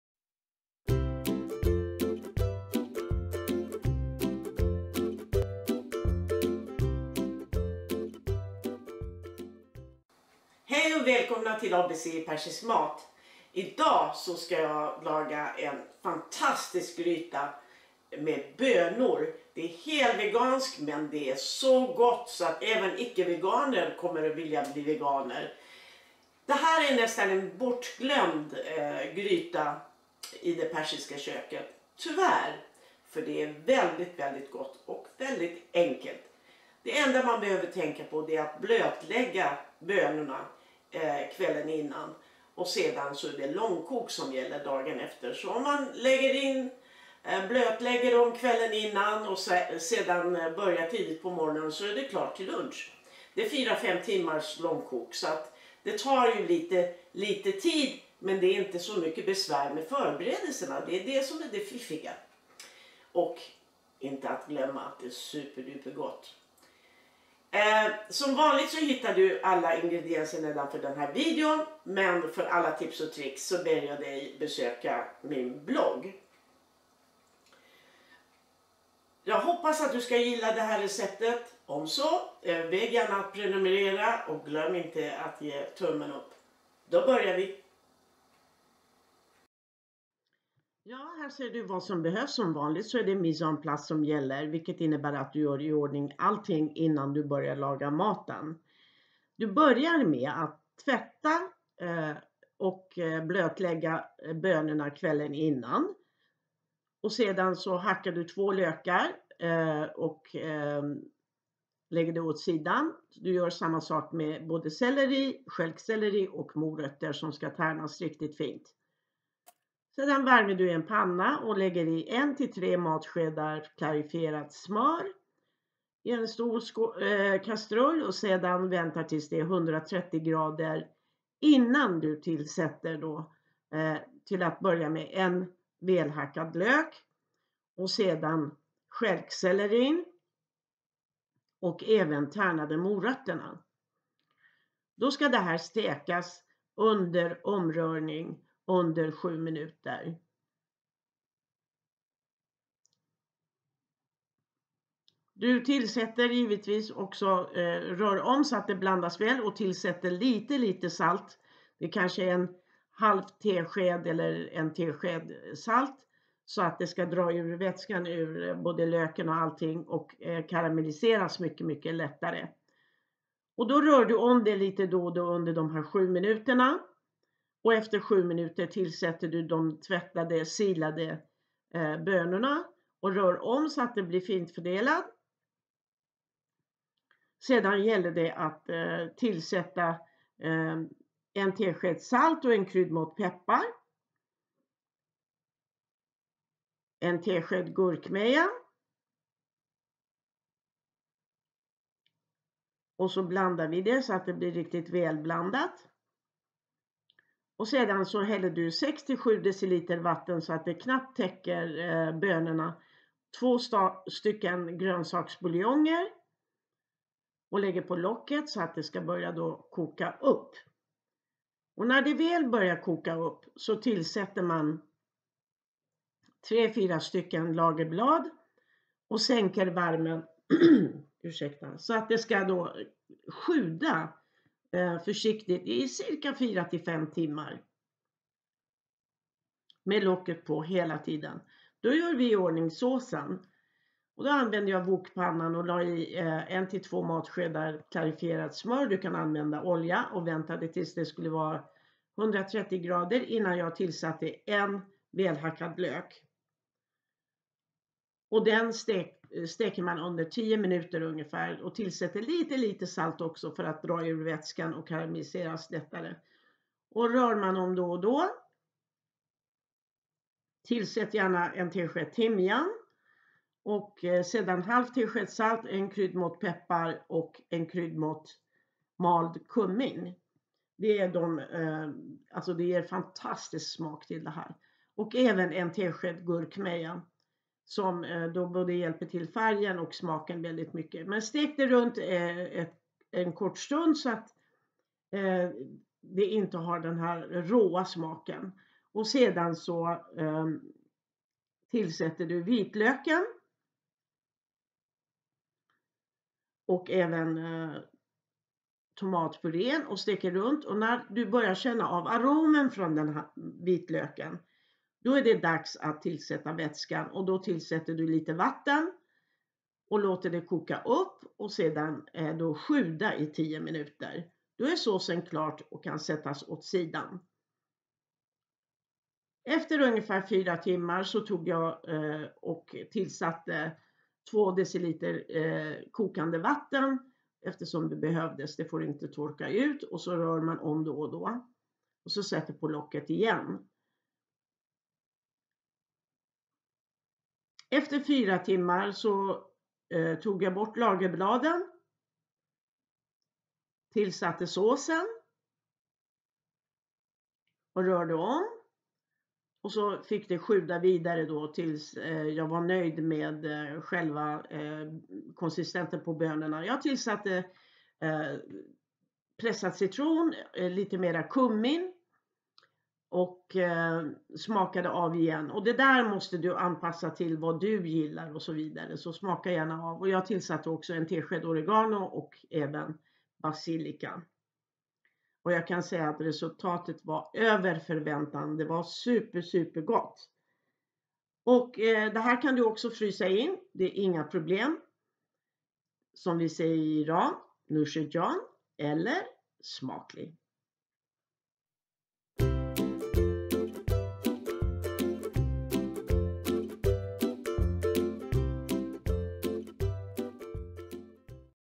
Vegansk böngryta
En fantastisk mustig vegansk böngryta som är nästan bortglömd i det persiska köket.
Du får en lista över ingredienserna här men för alla tips och tricks behöver du nog besöka min blogg

Ingredienser (6 portioner)
3 dl black-Eye bönor
3 dl kidneybönor
2 st. gula lökar
2 – 3 st. morötter
3 st. selleristjälkar
1.5 tsk salt
1 krm svartpeppar
1 tsk gurkmeja
3–4 st. lagerblad
2 st. grönsaksbuljonger
6 – 7 dl vatten
2 msk klarifierad smör eller olja
Såsen
1–2 msk klarifierad smör eller olja
1 gul lök
1 tsk timjan
0,5 tsk salt
1 krm svartpeppar
1 msk mald kummin
1 tsk gurkmeja
3 st. vitlöksklyftor
2 msk tomatpuré
2 – 3 dl vatten
2 msk pressad citron
Övriga smaksättningar
1 tsk torkad oregano
1 tsk torkad basilika
Noshe Jan!